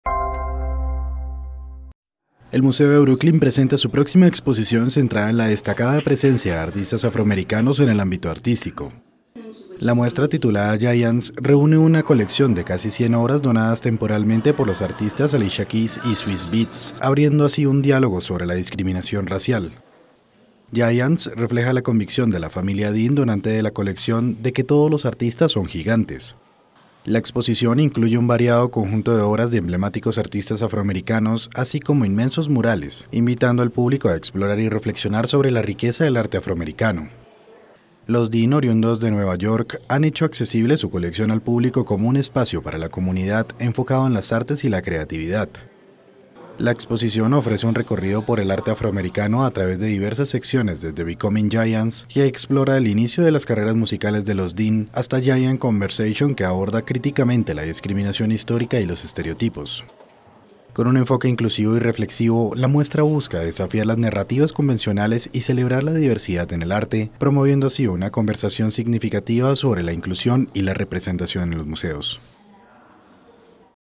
"Giants", la exhibición que reivindica el arte afroamericano en Estados Unidos
Nueva York, 6 feb (EFE).- El museo de Brooklyn reivindica en su próxima exposición la presencia de artistas afroamericanos en los museos y abre una "gran" conversación sobre la discriminación racial a través de una colección de casi cien obras donadas temporalmente por los artistas Alicia Keys y su esposo Swizz Beatz.
'Giants' ('Gigantes') es el título del conjunto de 98 obras que forman parte de la donación de la familia Dean (el apellido real del productor de hip-hop y DJ Swizz Beatz, Kasseem Dean) y que podrán verse a partir de este sábado 10 de febrero y hasta el 7 de julio en el interior del museo neoyorquino.
IMÁGENES: ÁNGEL COLMENARES.